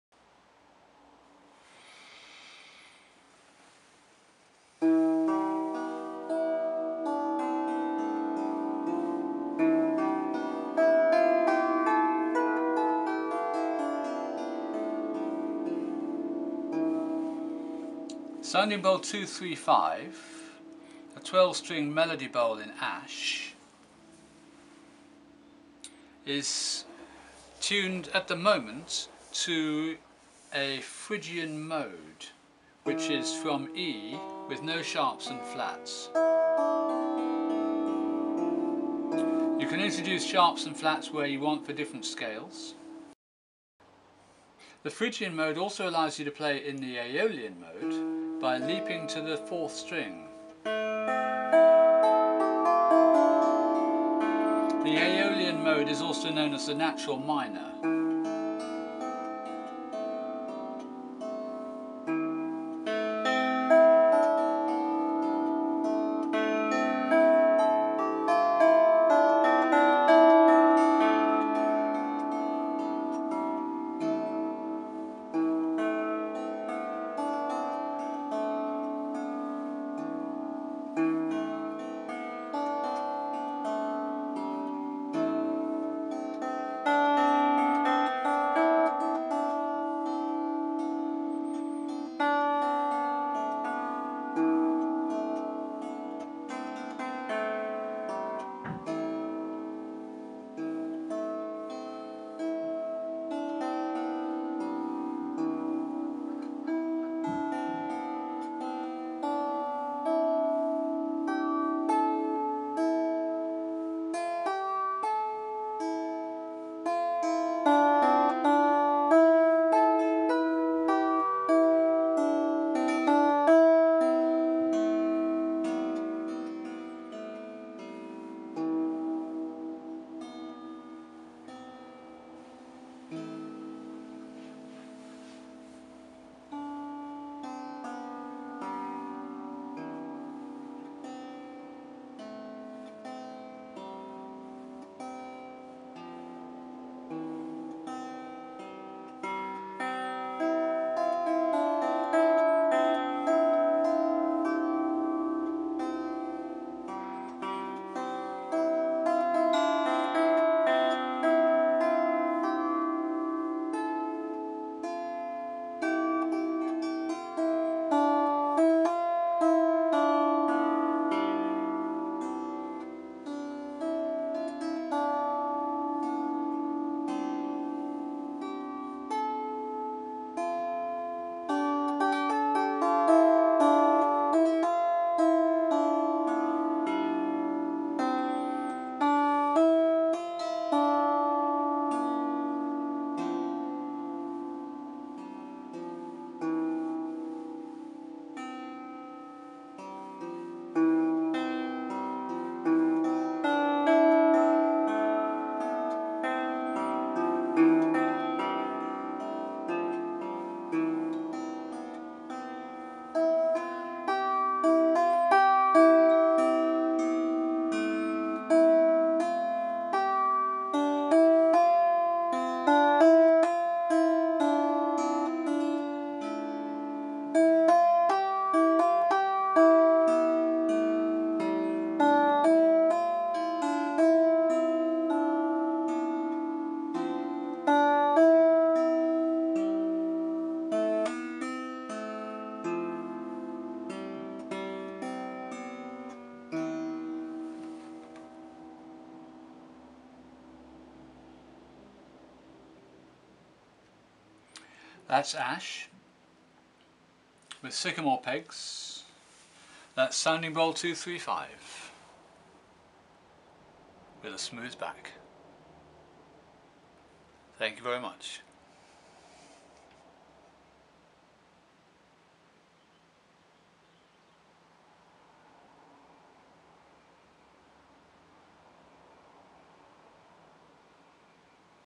Melody Bowl w. 12 strings. ~ #235 in Ash.wmv
The 12 string Melody bowl is typically light and slender to give it a bright, strong casting power while being light and easy to hold up for performance.
The music begins with scales and arpeggios in the Phrygian mode and moves on to tunes and improvisation in the Aeolian mode.